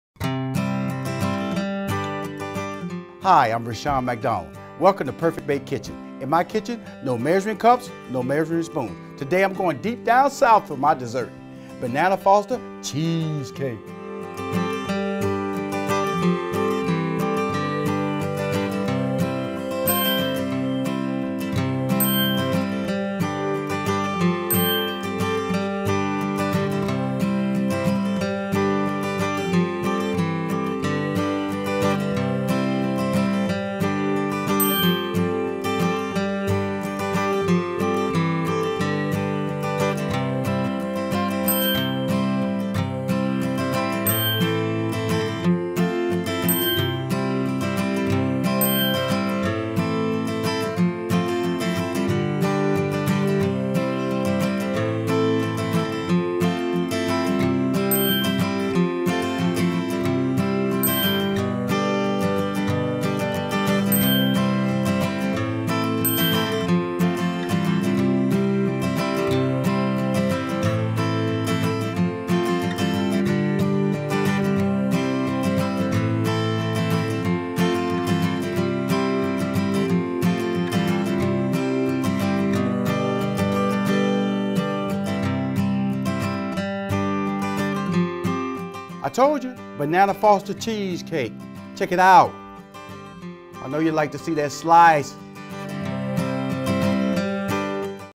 Banana Foster Cheesecake by Rushion McDonald
Watch Rushion show you how to make No Bake Marshmallow Apple Pie with Perfect Bake! Get the recipe for your own Perfect Bake